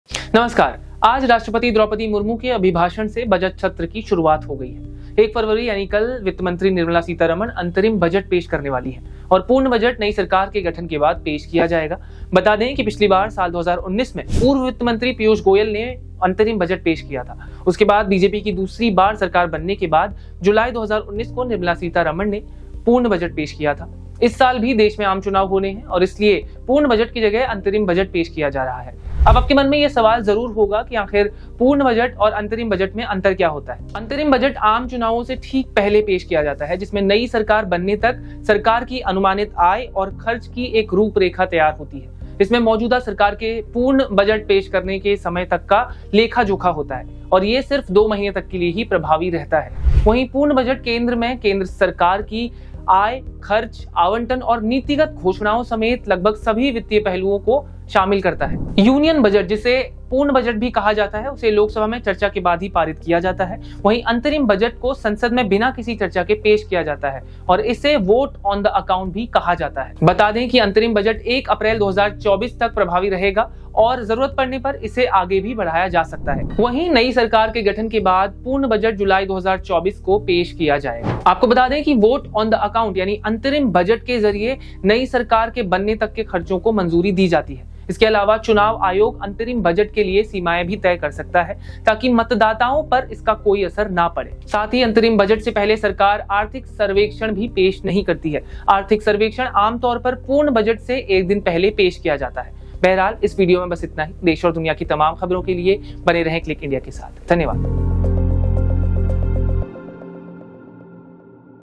Interim Budget 2024: अंतरिम बजट और यूनियन बजट में क्या अंतर है | Interim Budget vs Union Budget
इस बार भी देश में आम चुनाव होने हैं. और इसलिए पूर्ण बजट की जगह अंतरिम बजट पेश किया जा रहा है…देखिए क्या अंतर है Interim Budget और Union Budget में